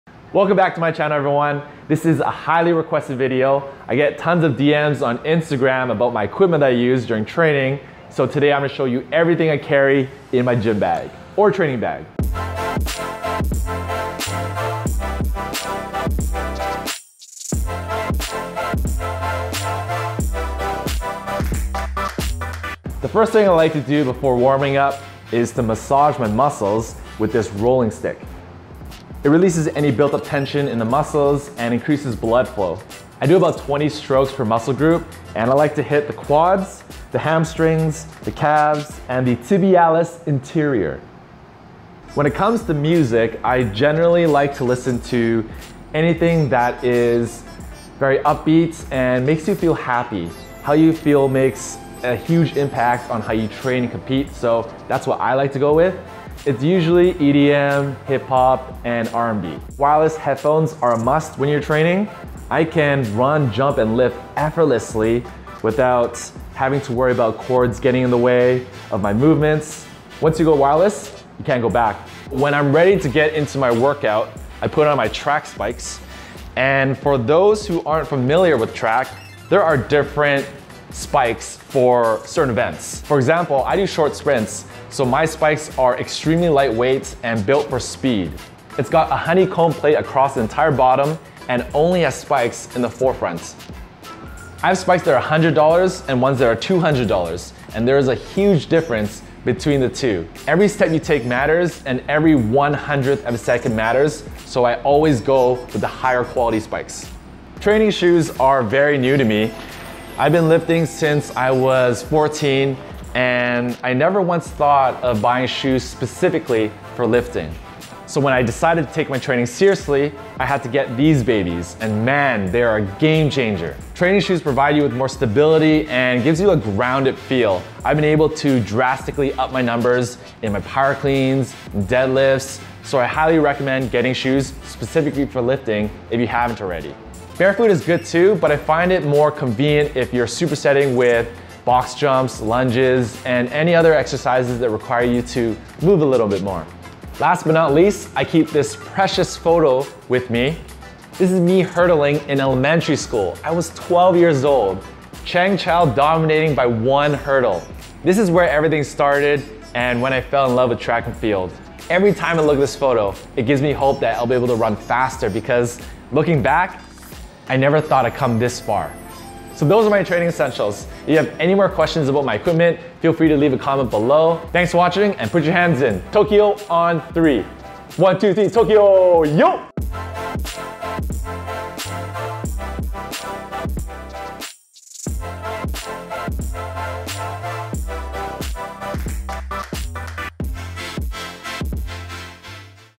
What's In My Gym Bag?
Here are my 5 essentials I bring to every workout: The Stick, Jaybird RUN, Nike Superfly Elite, Nike Metcon 3, and a photo of me hurdling for the first time.

If you're reading this, yell "TOKYŌ YO!" in the comments!